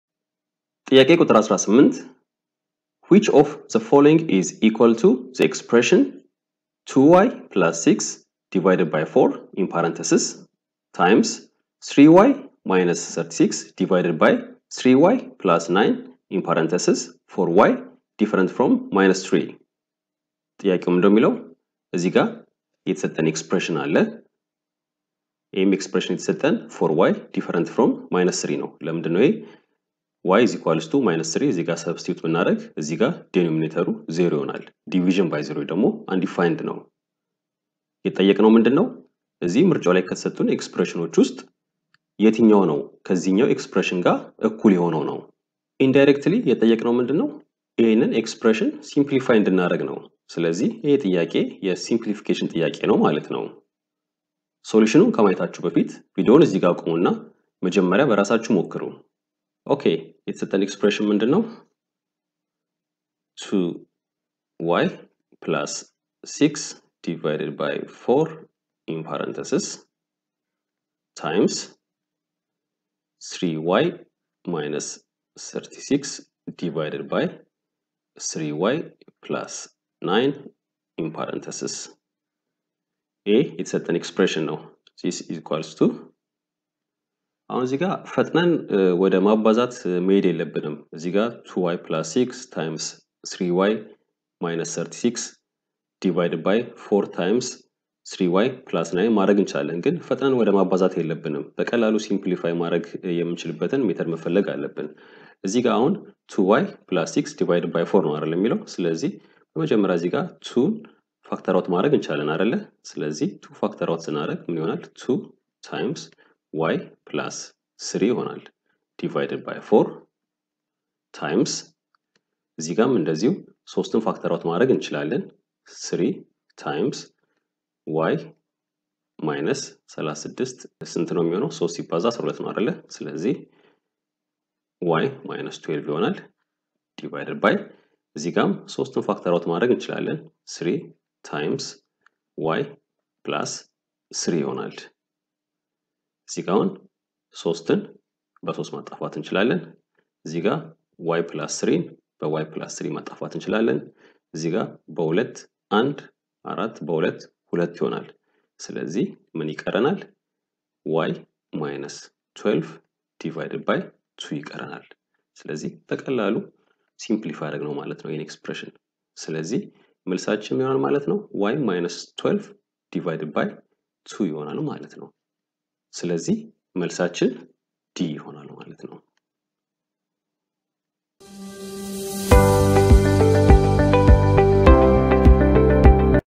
2013/14 Ethiopian Secondary School Leaving Certificate Examination Scholastic Aptitude Test- Q18/25
This question is from Ethiopian Secondary School Leaving Certificate Examination(ESSLCE) Scholastic Aptitude Test Quantitative Reasoning Section - Tikimit  2013/14 (November 2021)

Which of the following is equal to the expression ((2y+6)/4)x((3y-36)/(3y+9)) for  ≠ - 3 ?